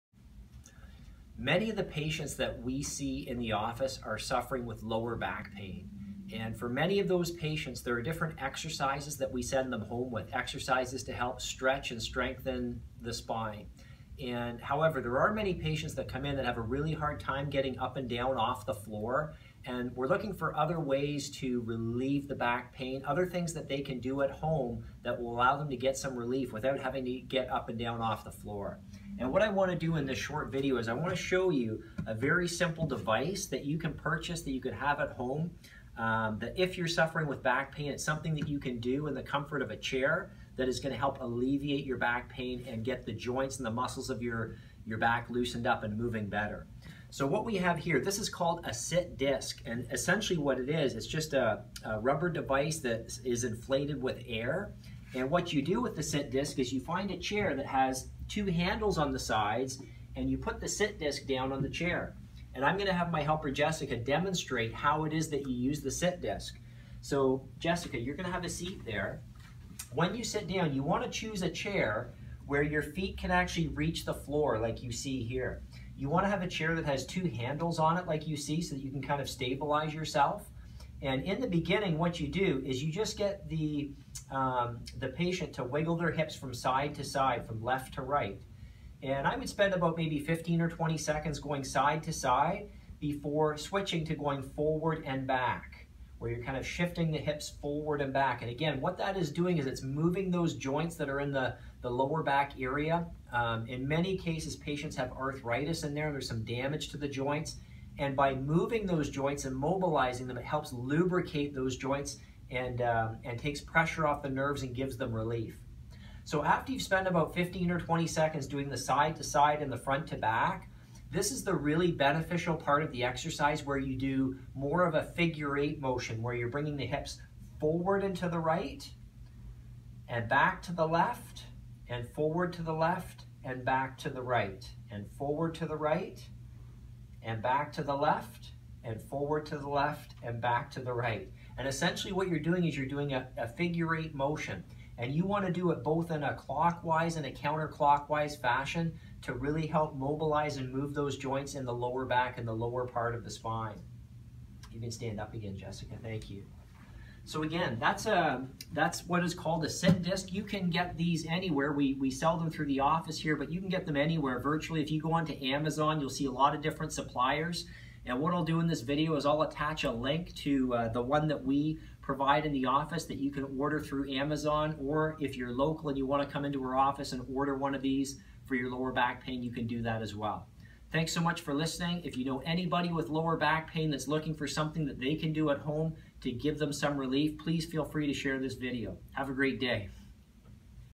Low Back Pain Relief Device You Can Use at Home
For those that have a hard time getting up and down to do stretching exercises for lower back pain, the sit disc is a really great way to get your spine moving and relieve pain from a seated position.